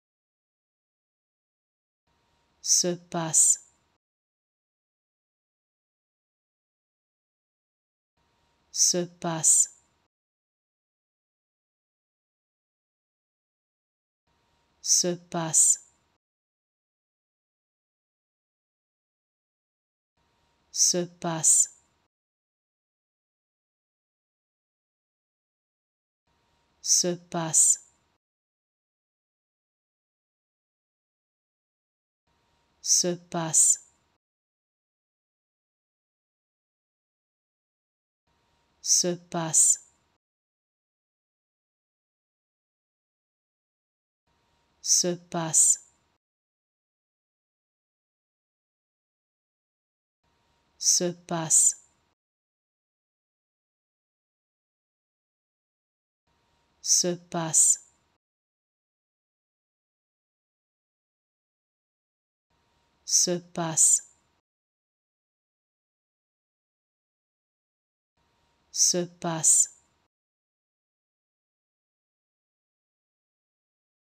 How to Pronounce ''Se passe'' Correctly in French
Learn how to say and properly pronounce ''Se passe'' in French with this free pronunciation tutorial. Apprenez à prononcer des mots en français grâce à nos tutoriels.